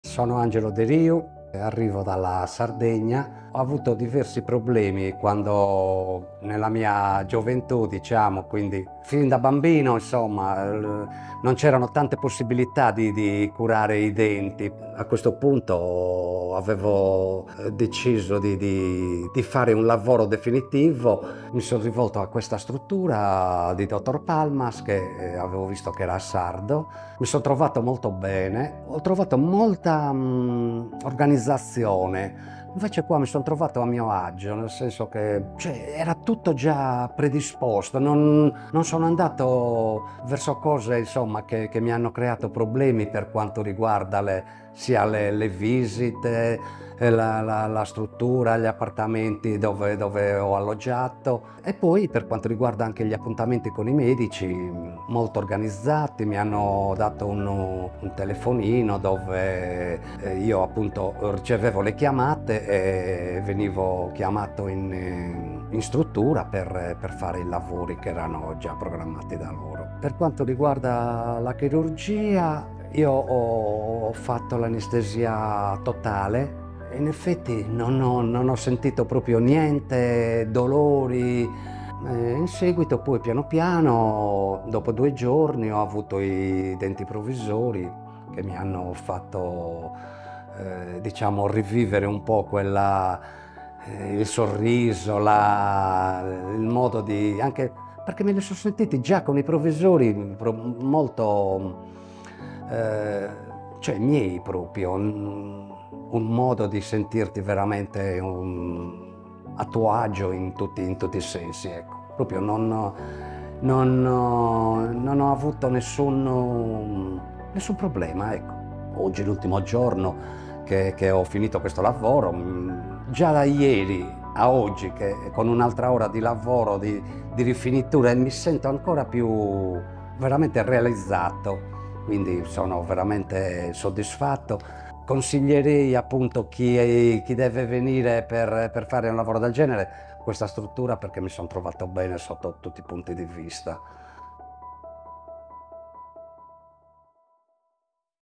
Angelo: La Scelta Definitiva per un Sorriso Perfetto
Mi chiamo Angelo De Riu e arrivo dalla Sardegna. Fin dalla gioventù ho avuto problemi dentali, poiché all'epoca non c'erano molte possibilità di curare i denti. Finalmente ho deciso di fare un lavoro definitivo e mi sono rivolto alla clinica del Dottor Palmas, attirato dal fatto che lui, come me, è sardo. L'esperienza è stata fantastica.

Ho trovato una struttura estremamente organizzata: tutto era già predisposto, dagli appartamenti alle visite, e non ho dovuto preoccuparmi di nulla. Mi hanno fornito anche un telefonino per le chiamate e gli appuntamenti in clinica, un servizio davvero impeccabile. La chirurgia è stata un successo, non ho sentito alcun dolore grazie all'anestesia totale, e due giorni dopo ho già ricevuto i denti provvisori, che mi hanno fatto riscoprire il sorriso.

Ora, dopo l'ultima rifinitura, mi sento finalmente realizzato. Consiglio vivamente questa struttura a chiunque abbia bisogno di un intervento dentale importante, perché mi sono trovato bene sotto tutti i punti di vista.